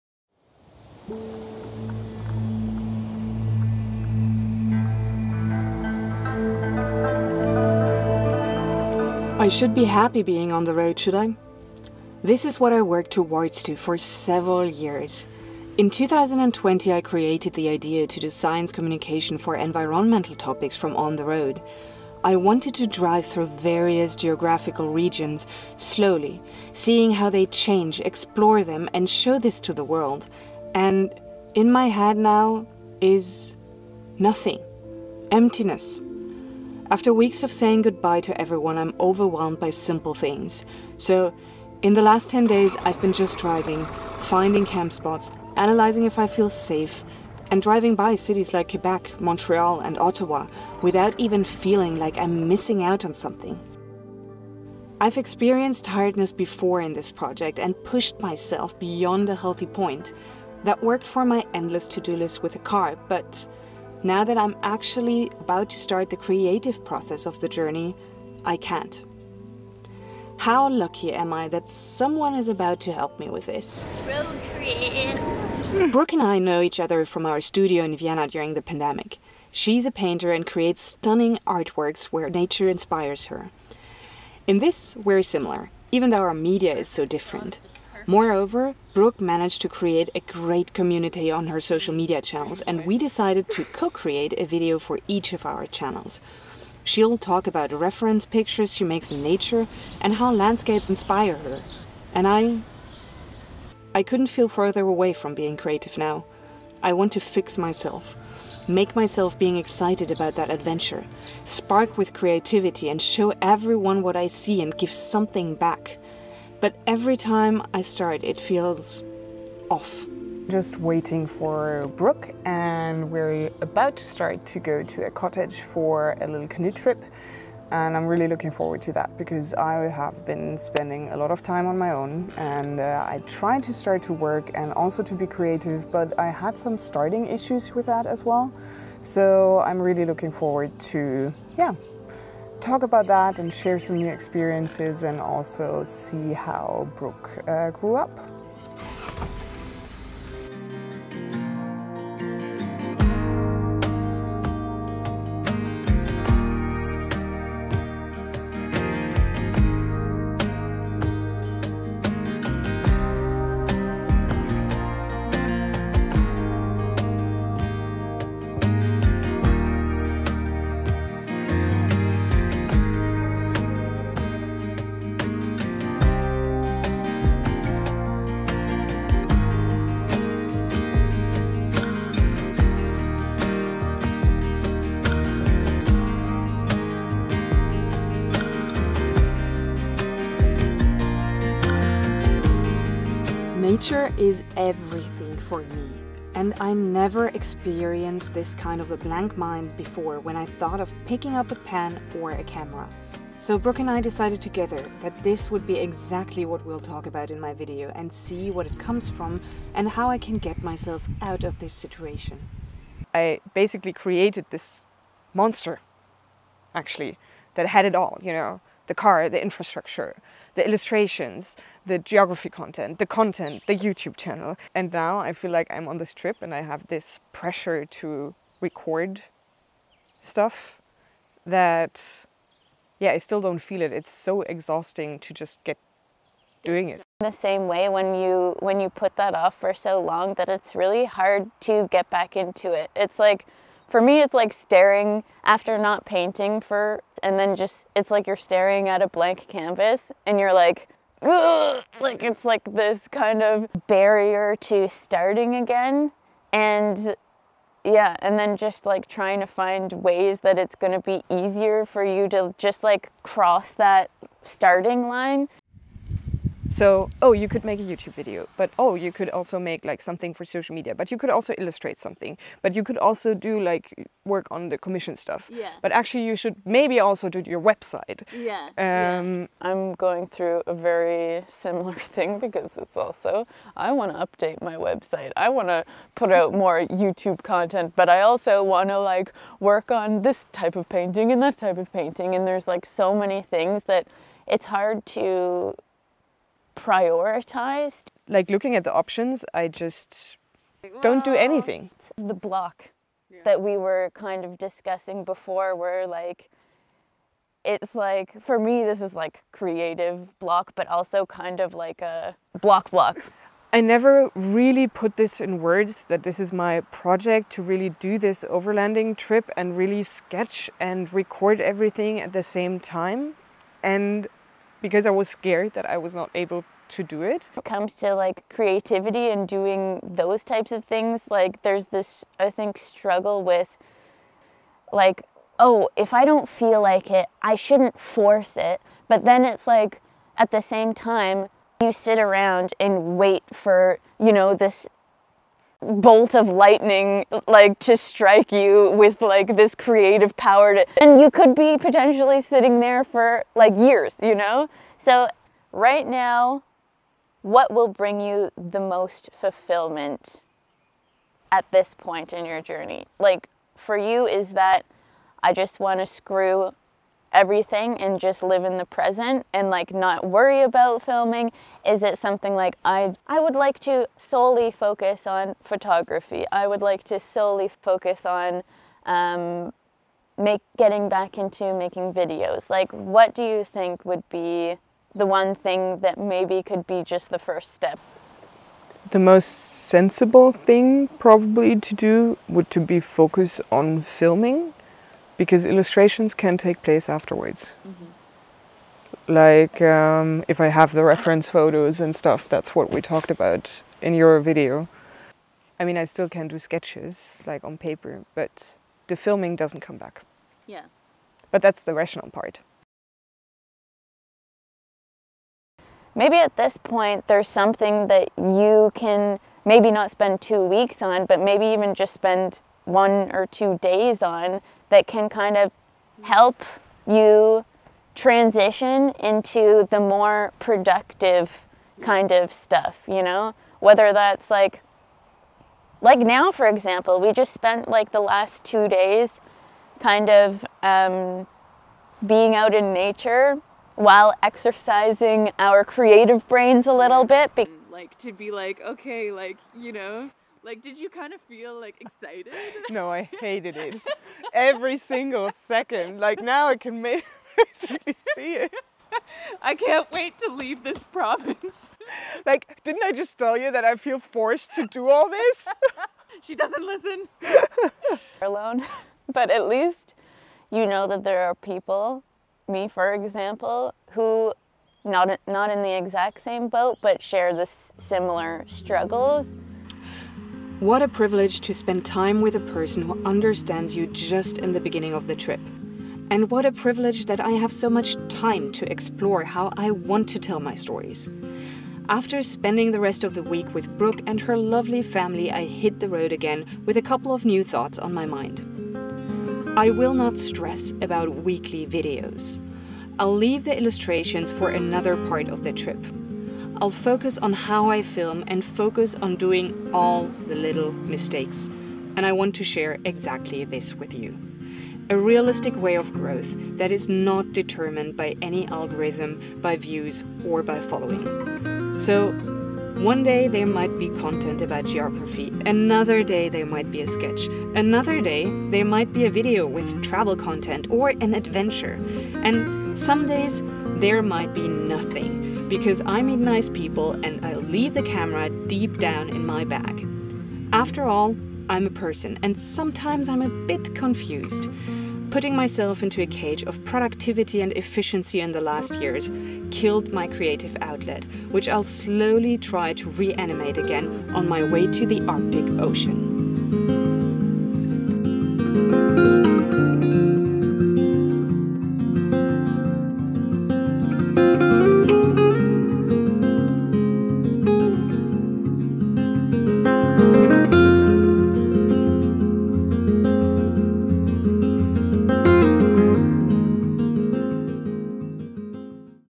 Brooke Cormier helps me out - creative block while starting overlanding.
Sometimes life just goes a different path with you. For the last four years an idea grew in my head to combine a science communication project with an overlanding journey along one of the longest roads of the world - the Panamerican Highway. The plan: See how landscapes change, document it, find interesting stories about geography and nature and sharing them with videos and illustrations!

Burns out that this plan is not so easy. Now that I’m on the road my mind is blank. I’m lucky that one of my first stops on the Transacandian Highway is a visit at Brooke Cormiers place, a Toronto based artist who creates stunning oil paintings and is a great inspiration to me. Brooke did not only take me to her cabin, on a canoe and a camping trip, she also listened, gave advice and lifted me up.

Creative blocks are something most of the people who live of their art experience. This is why it was important for Brooke and me to share this with you. One of the goals is to contribute to a more realistic perception of travel, remote working and self employment. Creative energy comes back, but for now I decided to continue taking it slow, focus on my clients and sharing on social media whenever it feels right!

The Land Cruiser and I continue our overlanding journey west in direction on the Rocky Mountains and then north to the Arctic Ocean - I'm curious what this project will still bring as I'm open to whatever happens on the trip now.

Thank you @b.cormier and the Cormier family for this wonderful week together in Ontario!